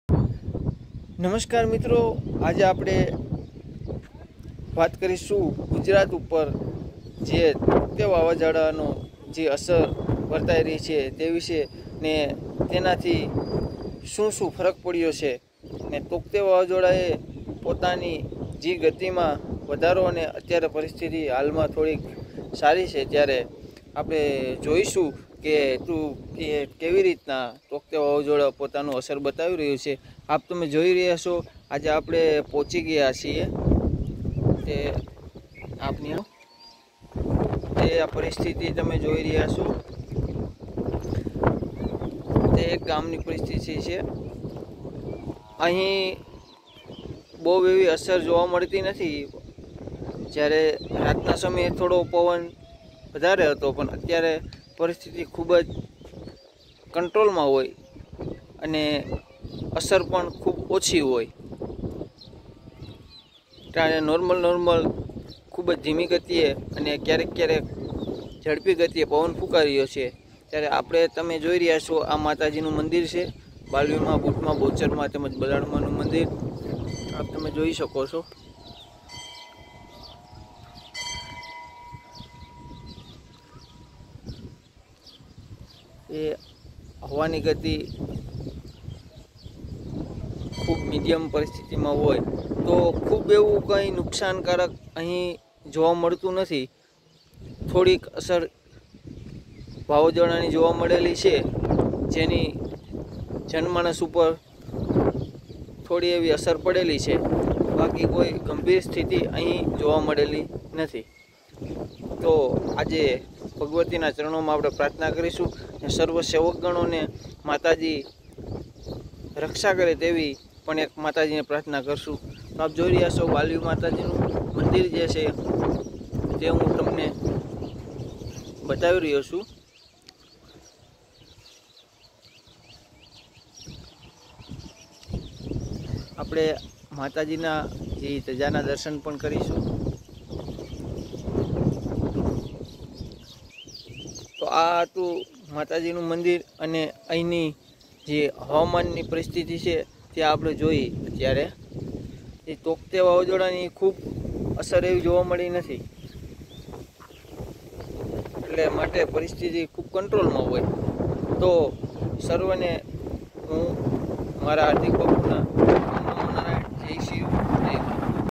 tauktae cyclone વાવાઝોડું gujarat update| વાવાઝોડાની અસર ,જોરદાર ટક્કર
tokte વાવાઝોડું gujarat update| વાવાઝોડાની અસર ,જોરદાર ટક્કર